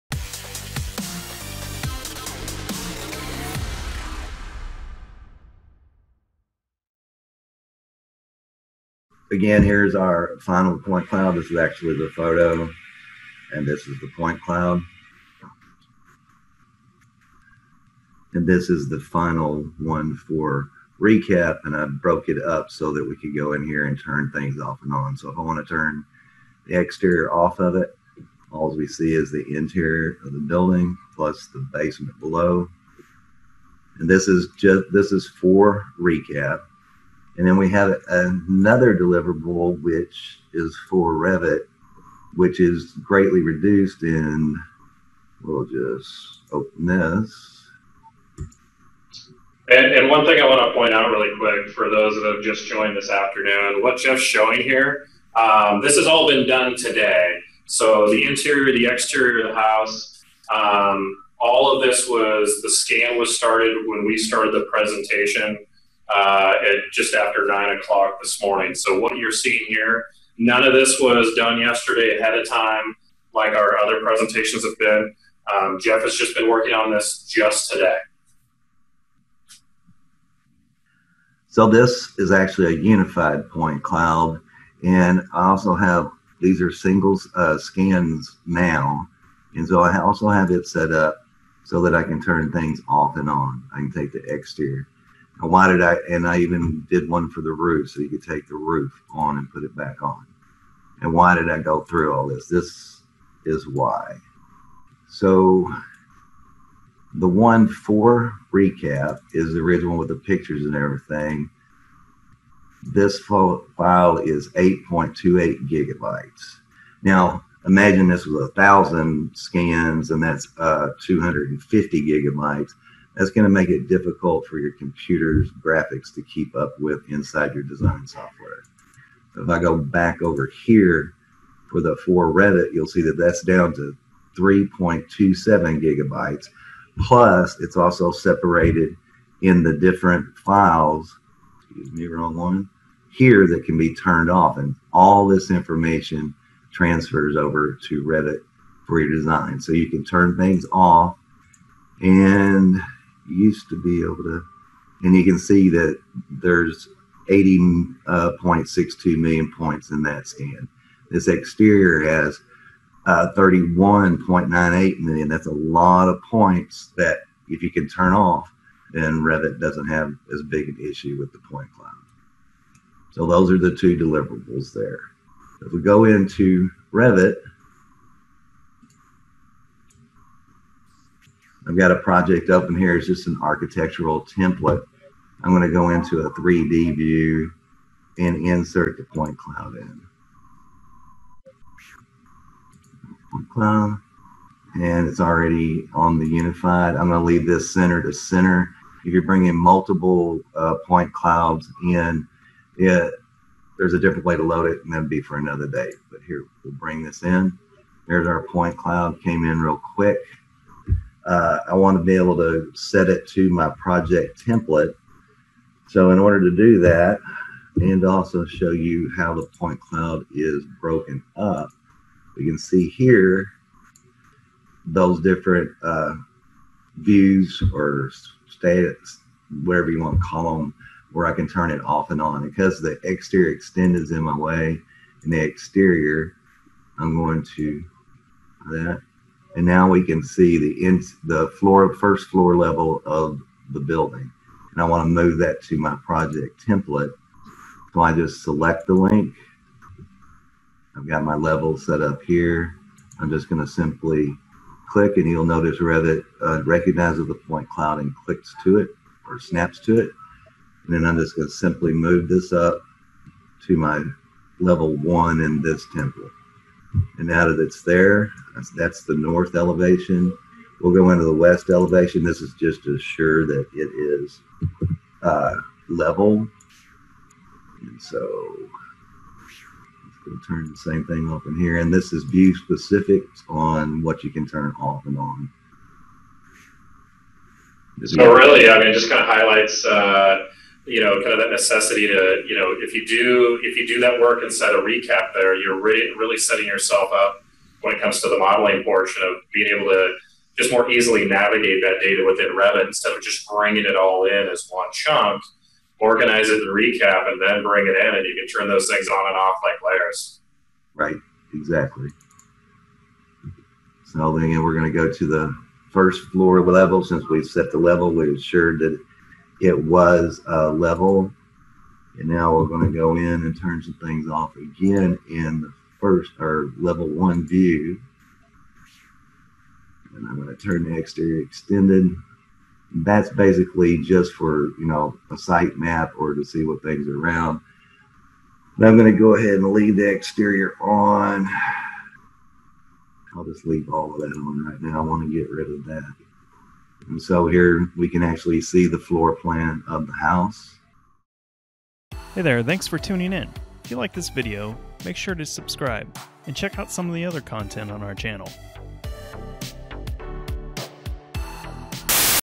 Formatting Point Cloud Data in ReCap Pro for Revit
In this video, we'll walk you through how to manage a point cloud in ReCap Pro in the most efficient way for modeling in Revit. This will help minimize issues, such as, lag or crashes, that come from managing large point clouds.